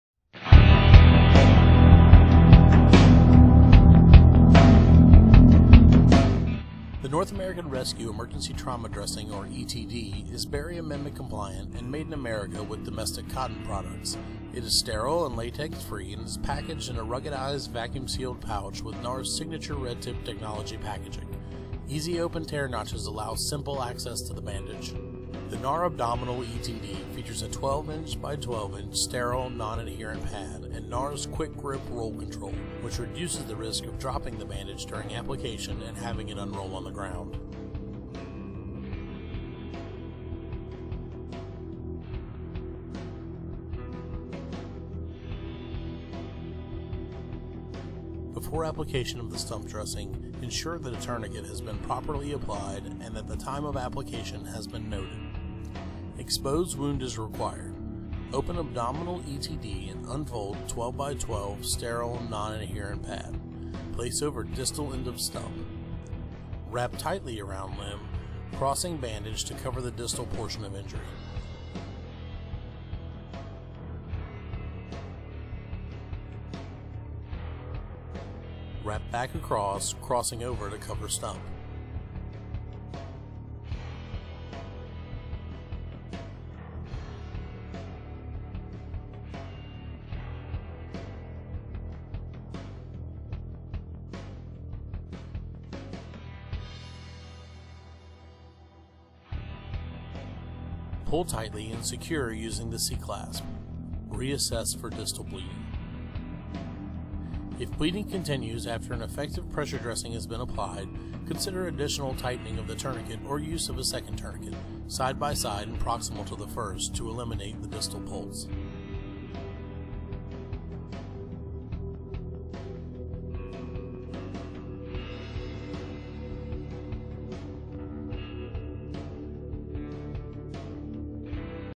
NAR Abdominal/Stump ETD- Stump Application
The Abdominal/Stump ETD™ from North American Rescue is an ideal for dressing abdominal eviscerations, traumatic amputations, burns and large pattern wounds. This ETD™ offers advanced wound care management on a larger scale and consists of a resilient elastic wrap equipped with an oversized 12 in. x 12 in. sterile non-adherent pad, durable securing device and innovative Quick-Grip™ Roll Control configuration. This combination facilitates a rapid, controlled, and extremely effective application process in the most demanding of conditions. In addition, its vacuum-sealed, low-cube packaging is not only compact, but also easy to open.